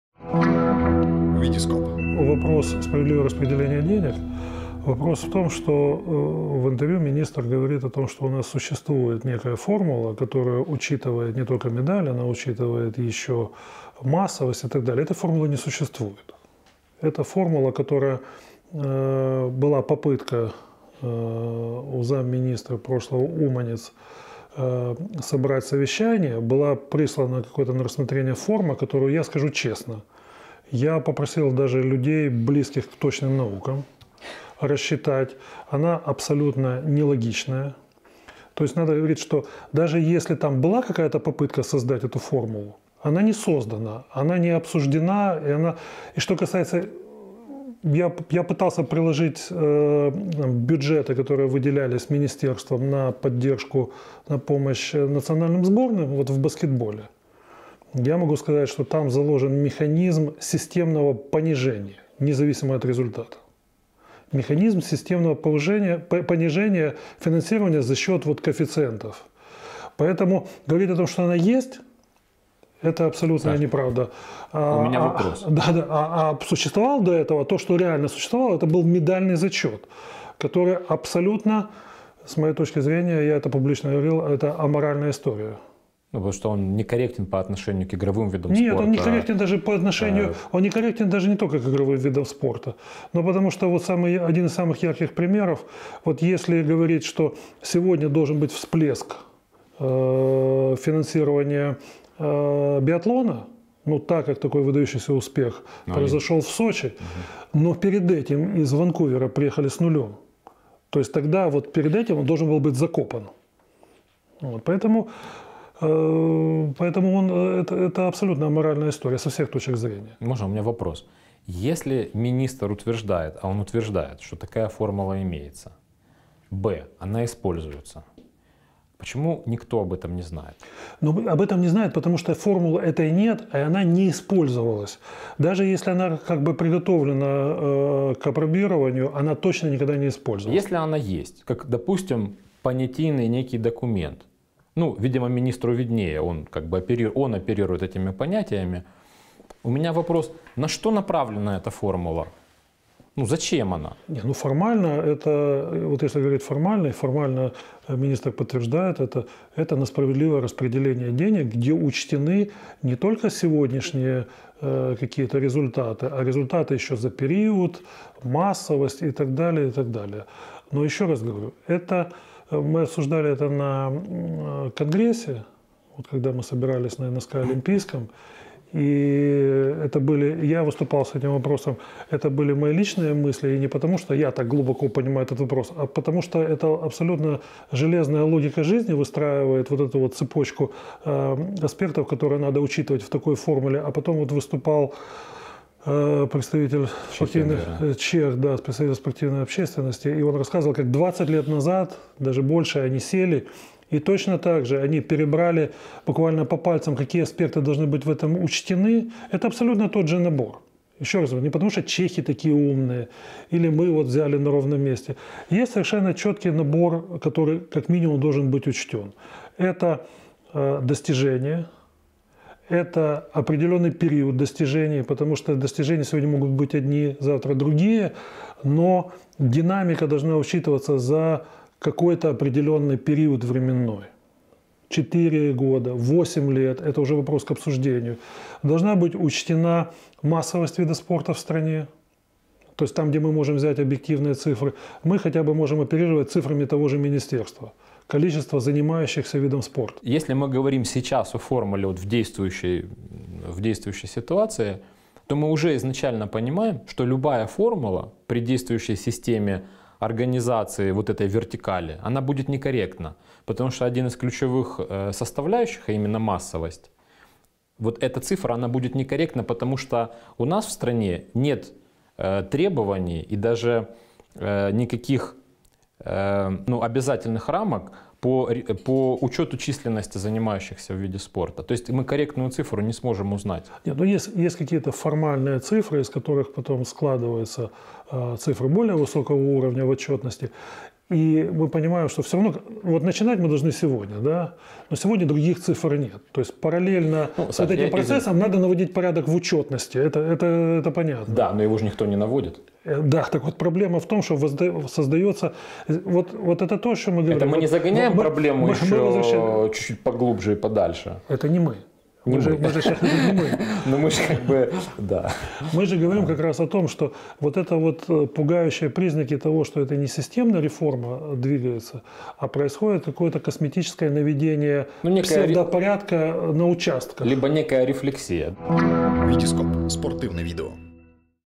Александр Ларин и Александр Кошевой про формулу финансирования
Генеральный секретарь Федерации баскетбола Украины Александр Ларин и президент Федерации спортивного права Украины Александр Кошевой рассуждают про формулу финансирования спорта в Украине.